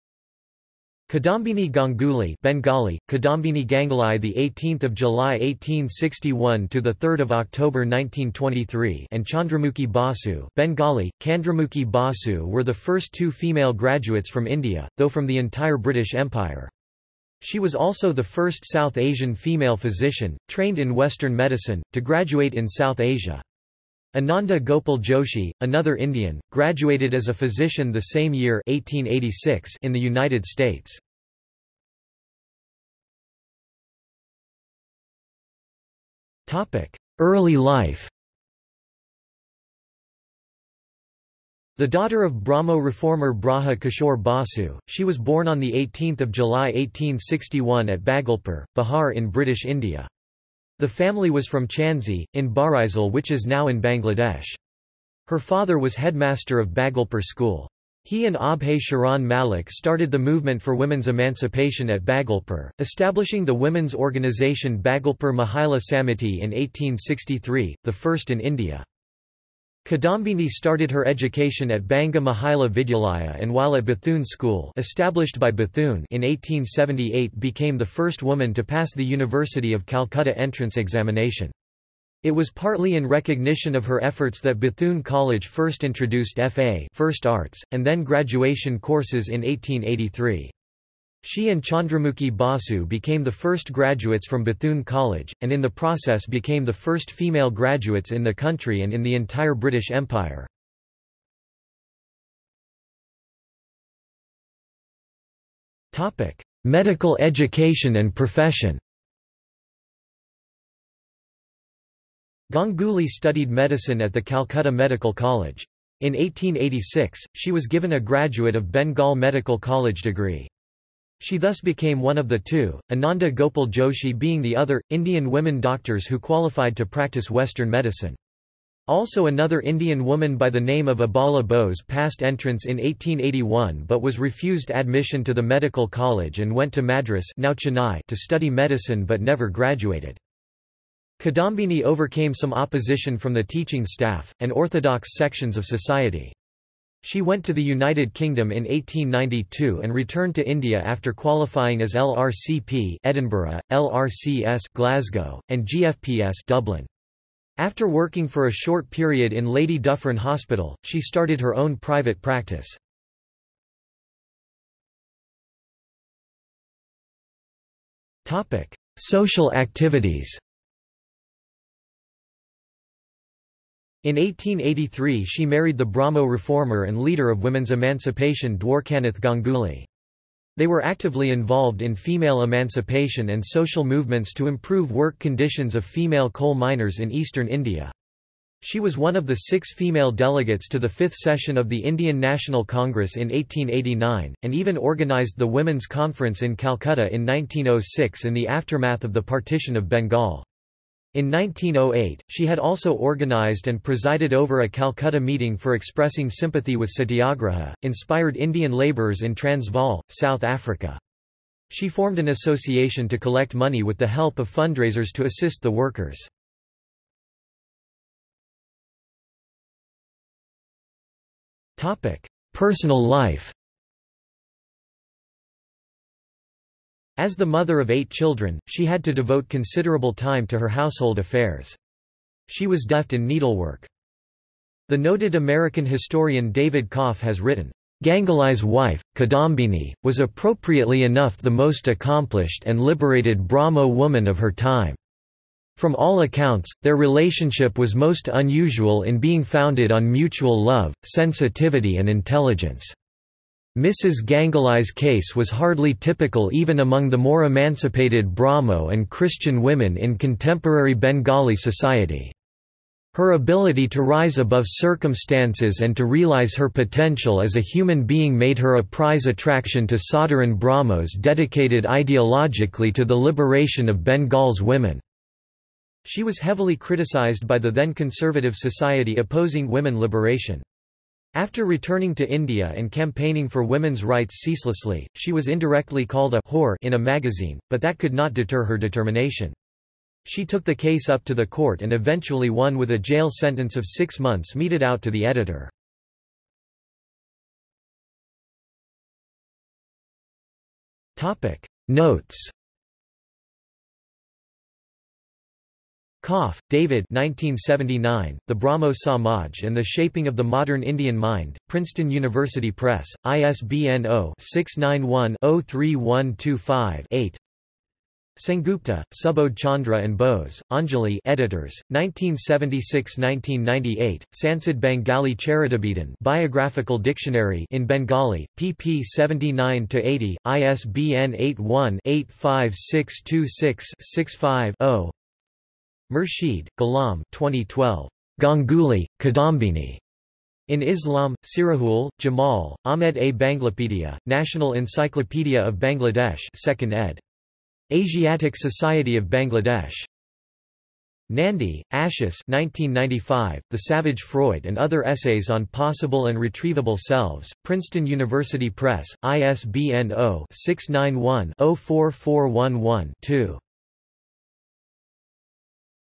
Kadambini Ganguly | Wikipedia audio article
This is an audio version of the Wikipedia Article:
Kadambini Ganguly

00:00:32 1 Early life
00:01:42 2 Medical education and profession
00:02:44 3 Social activities
00:03:38 4 Personal life
00:04:57 5 Notes

- Socrates

SUMMARY
Kadambini Ganguly (Bengali: কাদম্বিনী গাঙ্গুলি; 18 July 1861 – 3 October 1923) and Chandramukhi Basu (Bengali: চন্দ্রমুখী বসু) were the first two female graduates from India, though from the entire British Empire. She was also the first South Asian female physician, trained in western medicine, to graduate in South Asia. Anandi Gopal Joshi, another Indian, graduated as a physician the same year (1886) in the United States.